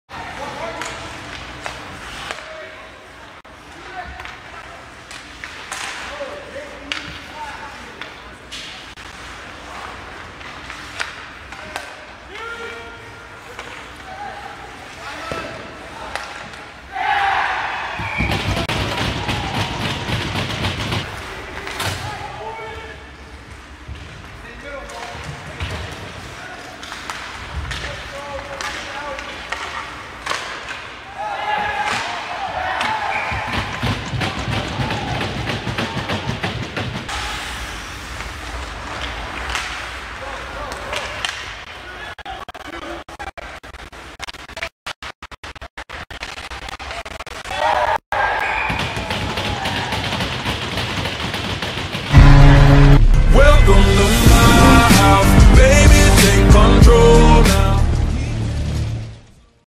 16U : Hitmen Showcase FJB vs. Syracuse Stars
Goals:
Bailey Kaiser (1) ASST: Michael Schmitt (1), Jason Alten (1) (PP)
Jack Conley (1) ASST: Jake Sylvia (1), Matthew Walters (1)
Jack Conley (2) ASST: Adam Hauptman (1)